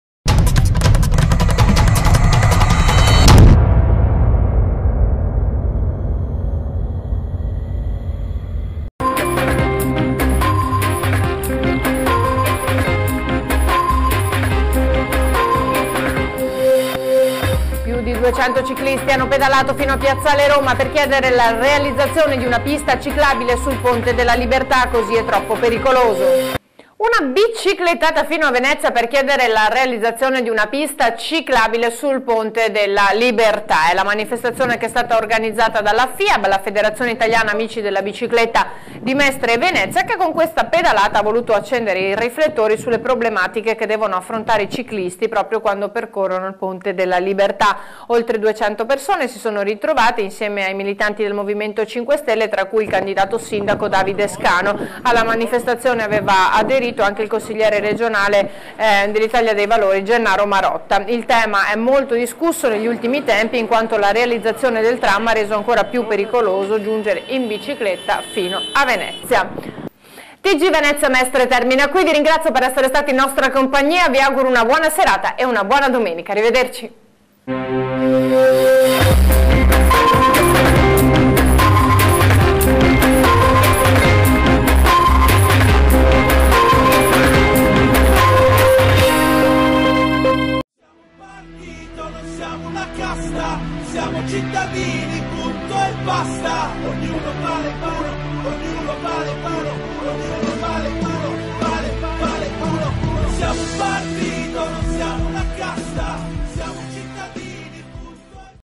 Servizio ReteVeneta 21.03.15 - Davide Scano partecipa a "Una pedalata per la pista ciclabile"
Servizio sulla manifestazione "Una pedalata per la pista ciclabile" a cui ha partecipato il nostro Candidato Sindaco Davide Scano